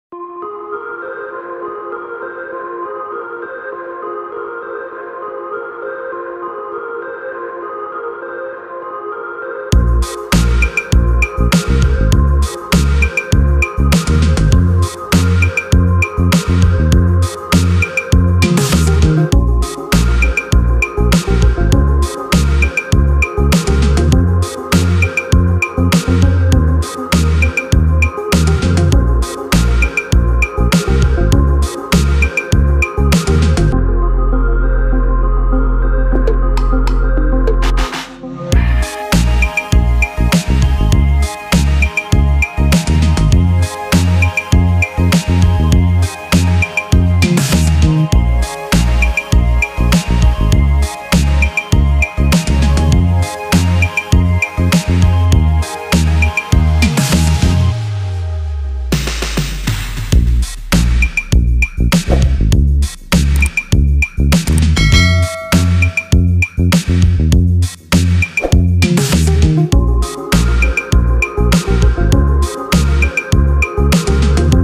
Cách Thêm Ghi Chú Vào Bài Đăng Và Reels Trên Instagram (Phương Pháp Mới)
Video Này Tôi Sẽ Chỉ Cho Bạn Cách Thêm Ghi Chú Vào Bài Đăng Và Reels Trên Instagram (Phương Pháp Mới) | Sẽ Chỉ Cho Bạn Quy Trình Dễ Nhất Để

Vì vậy, hãy Tiếp Tục Xem Toàn Bộ Video Mà Không Bỏ Qua Để Tìm Hiểu 2 Cách Thêm Ghi Chú Vào Bài Đăng Và Reels Trên Instagram (Phương Pháp Mới)

Add-Notes#On-Instagram#Reels